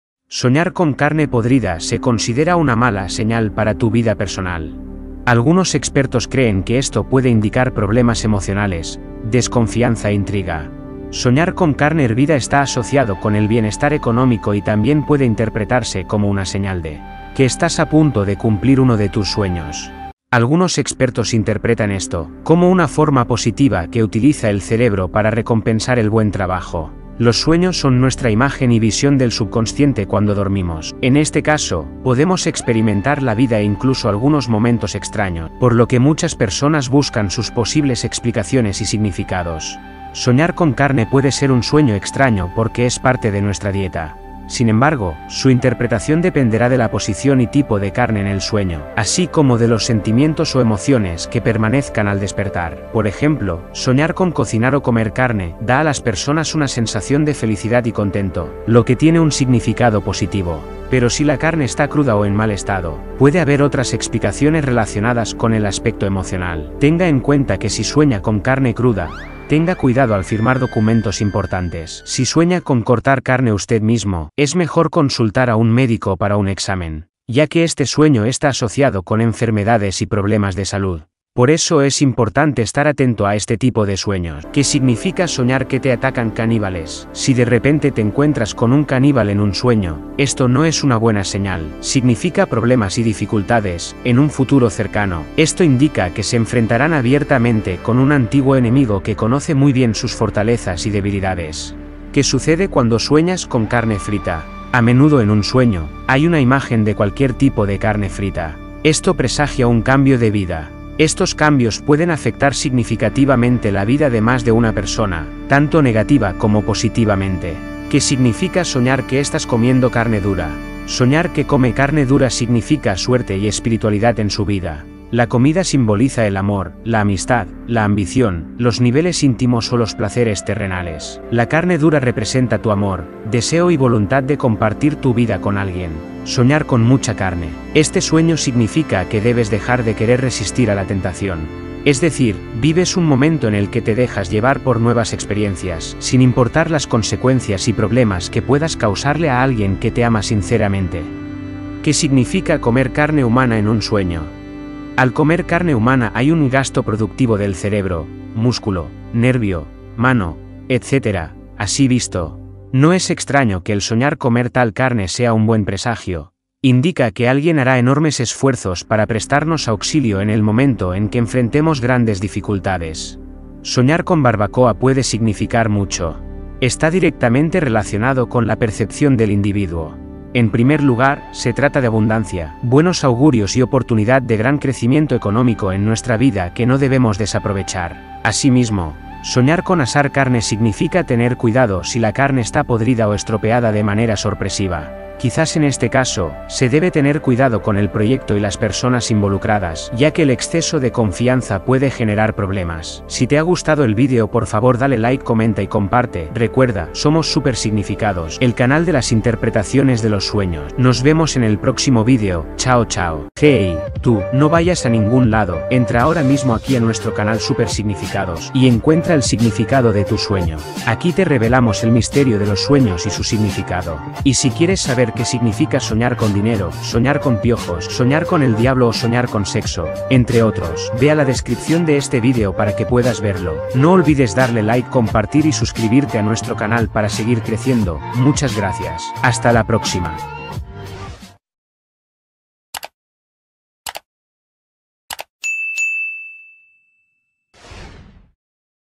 ¡QUE SIGNIFICA SOÑAR CON carne SIGNIFICADO Interpretación de los sueños 2023?
Soñar con carne puede tener varios significados dependiendo del contexto del sueño y de las emociones relacionadas con la carne en la vida del soñador. En general, la carne puede simbolizar la sustentación física y emocional, la energía y la fuerza. También puede representar deseos o necesidades instintivas o primitivas. Por otro lado, la carne también puede ser vista como un símbolo de la vida y la muerte, o como una metáfora para el cuerpo humano.